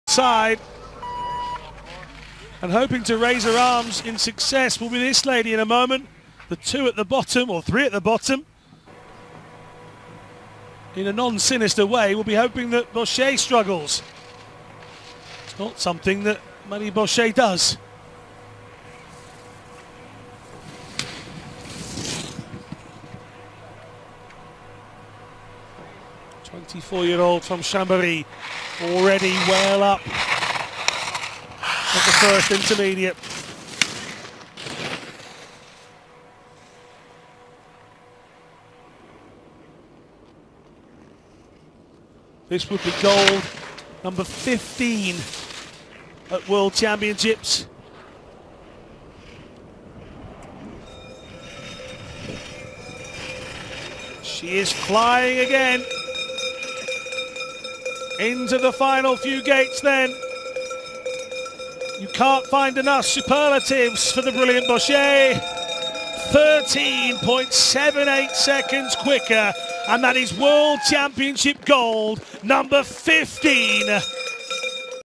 Marie Bochet Giant Slalom Run 2 | 2019 WPAS Championships
Run two for Marie Bochet (France) in the Women's Giant Slalom standing on day two of the 2019 World Para Alpine Skiing Championships in Kranjska Gora, Slovenia. Like and follow World Para Alpine Skiing on Facebook, Twitter and Instagram at: @ParaAlpine